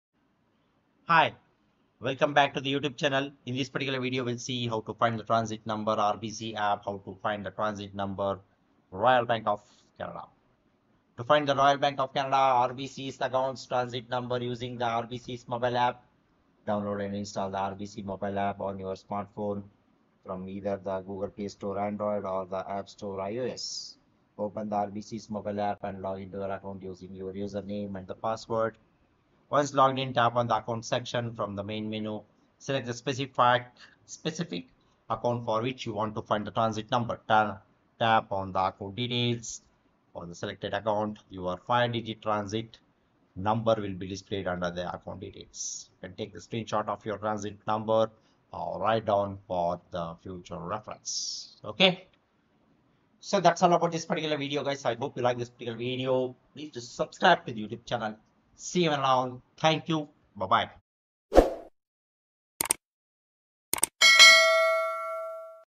How To Find Transit Number RBC App (How To Find Transit Number Royal Bank of Canada)
Today, we're going to show you How To Find Transit Number RBC App .This can be helpful in various situations, such as Finding the transit number in the RBC app streamlines banking, ensures accurate transactions, speeds up processes, enhances convenience, and improves financial management.

0:00 Introduction
0:12 Solution To Find Your Royal Bank Of Canada (RBC) Account's Transit Number Using The RBC Mobile App